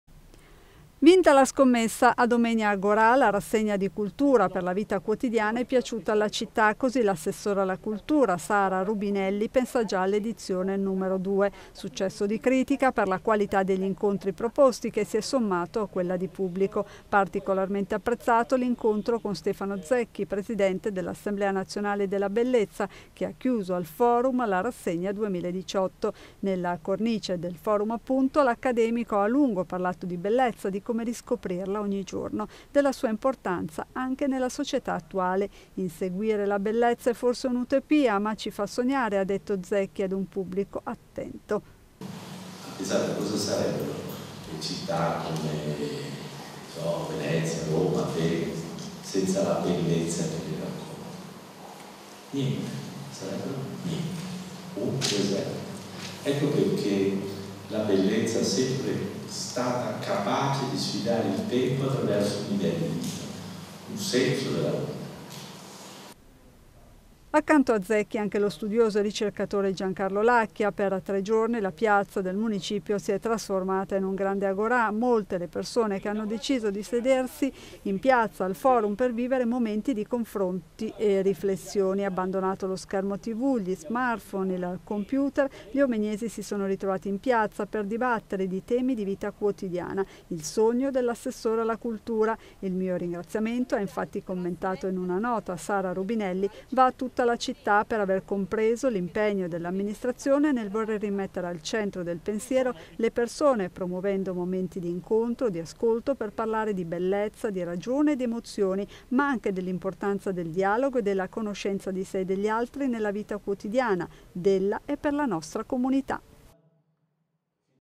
Agorà. Rubinelli: "Seconda edizione è una promessa alla città"
18 settembre 2018 - Il Comune di Omegna tira le fila dopo i dibattiti e le conferenze del passato fine settimana. Ora si pensa all'edizione 2019